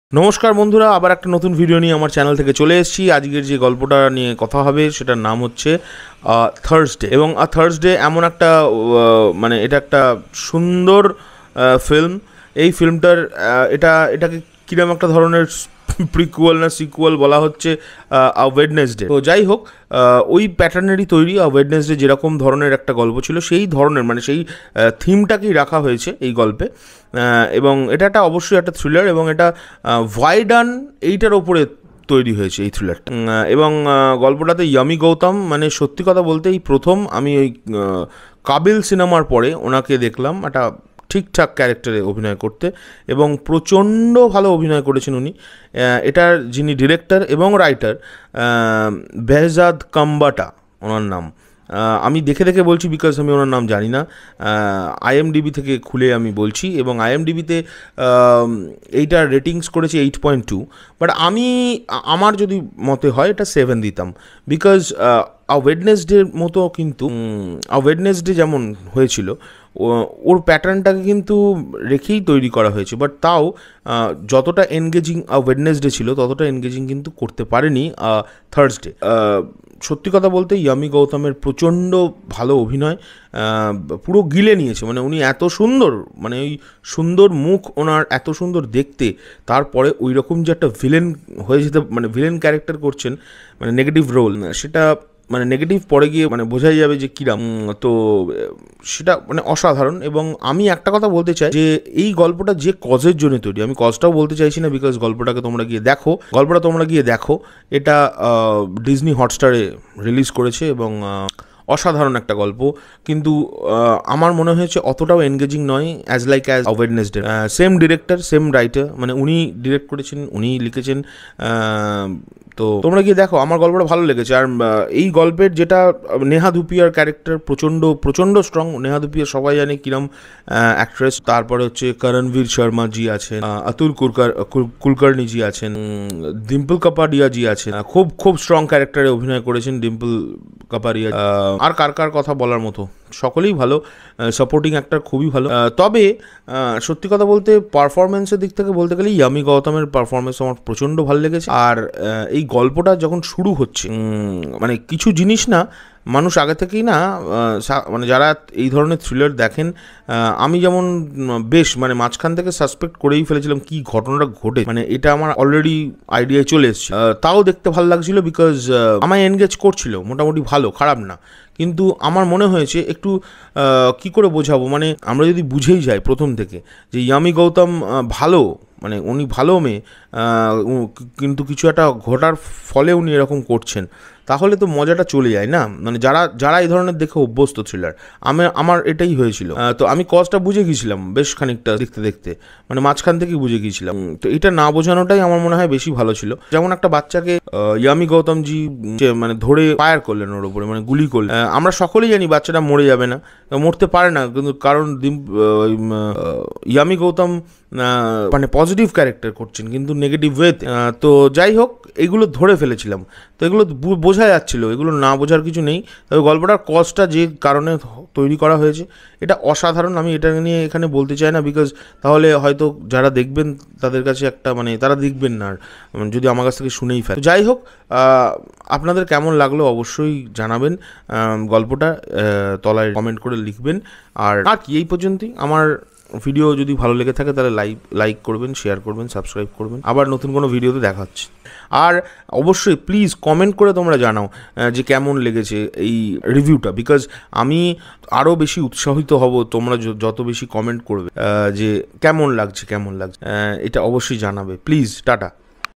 A Thursday 2022 | Review Bengali | Yami Gautam | Atul Kulkarni | Blue Monkey Films
A Thursday 2022 | Review Bengali |Yami Gautam

A Thursday is a 2022 Indian Hindi-language vigilante thriller film written and directed by Behzad Khambata. Ronnie Screwvala, Premnath Rajagopalan, jointly produced the film under the banners RSVP Movies and Blue Monkey Films. It stars Yami Gautam, Atul Kulkarni, Neha Dhupia, Dimple Kapadia, and Karanvir Sharma.